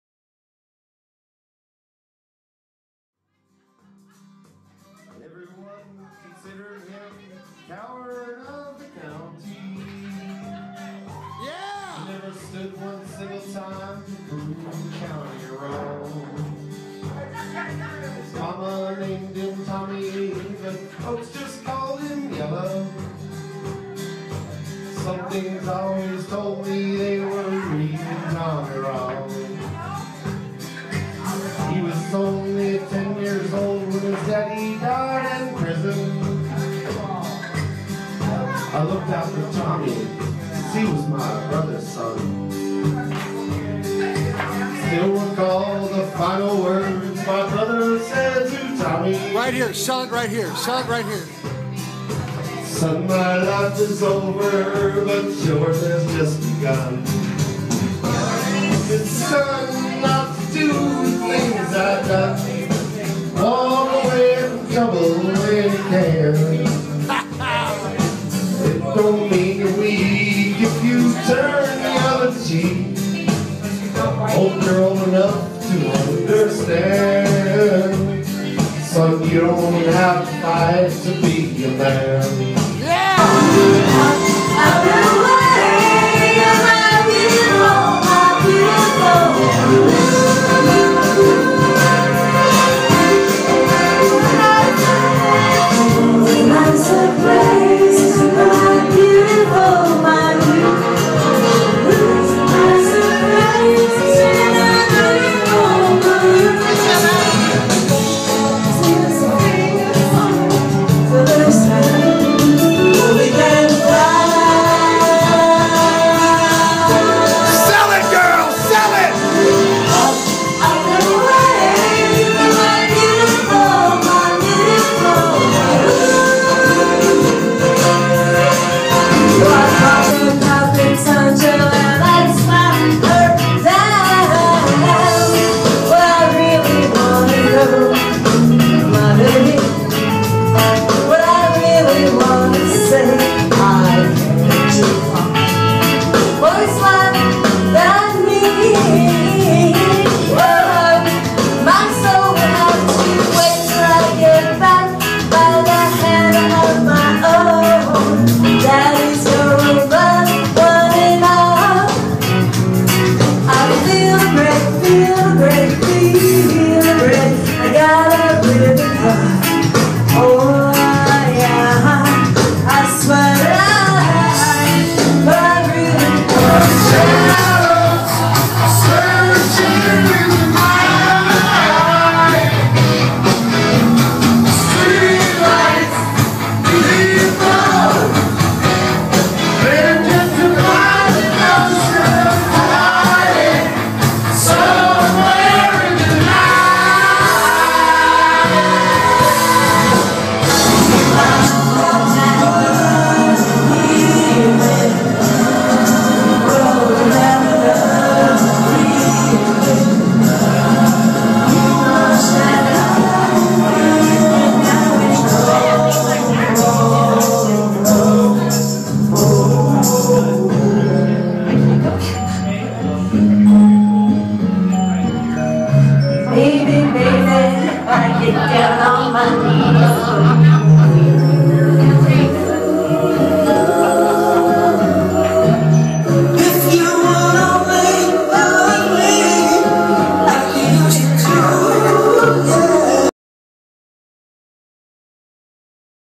Aussie Pub Karaoke
The Wednesday night karaoke at the Aussie is awesome! Come sing.